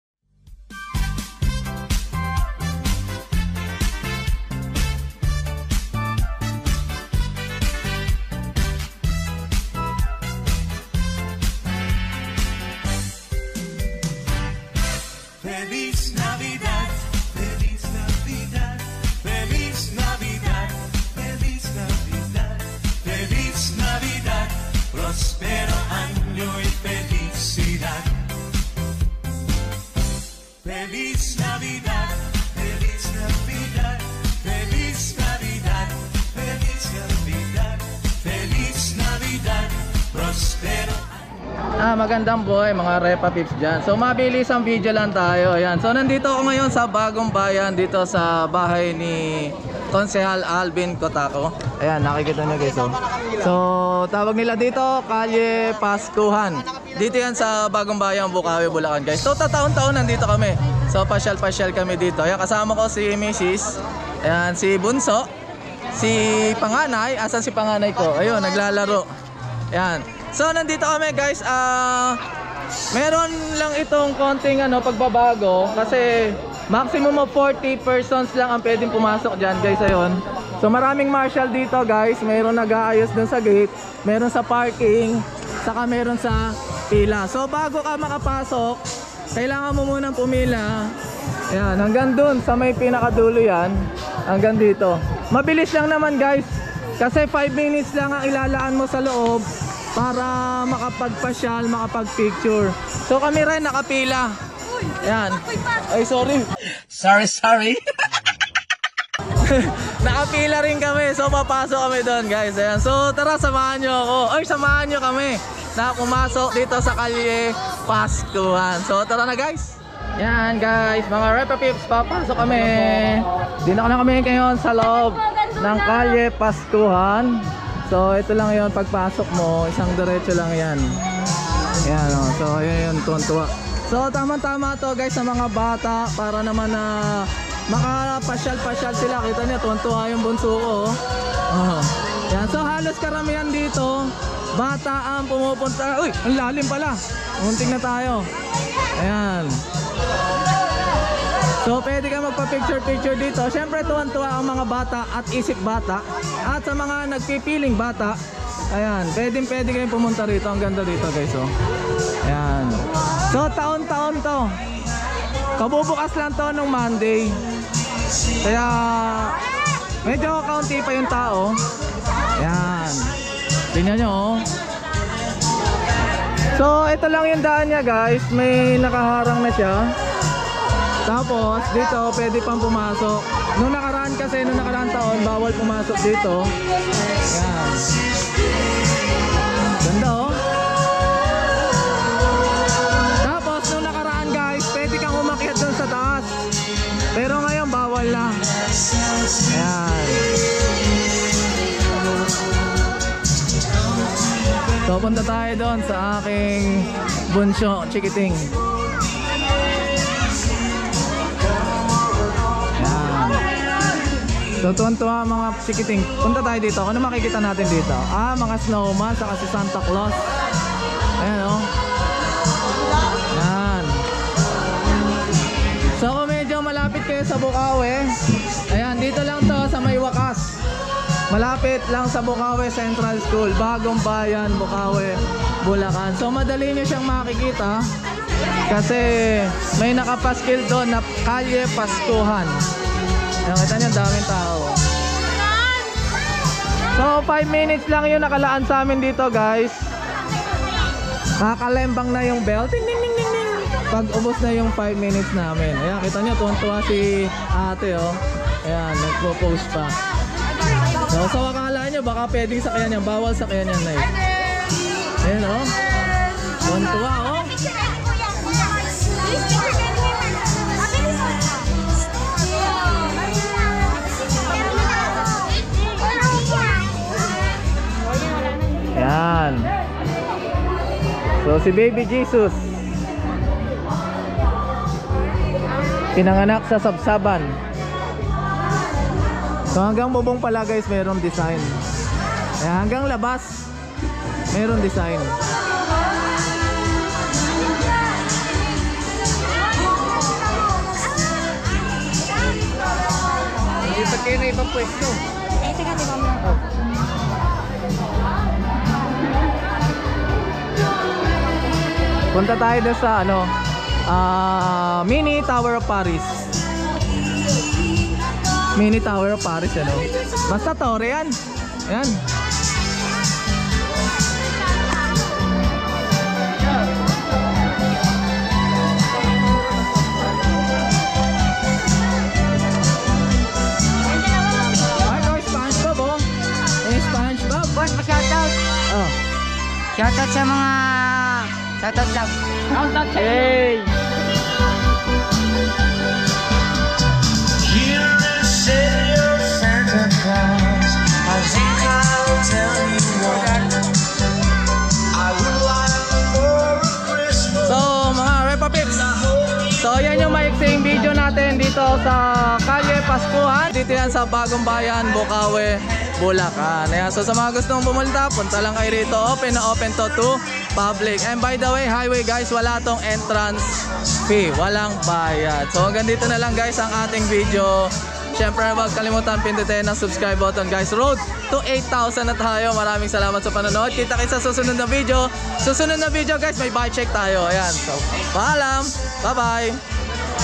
Kalye Paskuhan sa Bocaue Bulacan Bukas na sa Publiko
Bagumbayan Bocaue Bulacan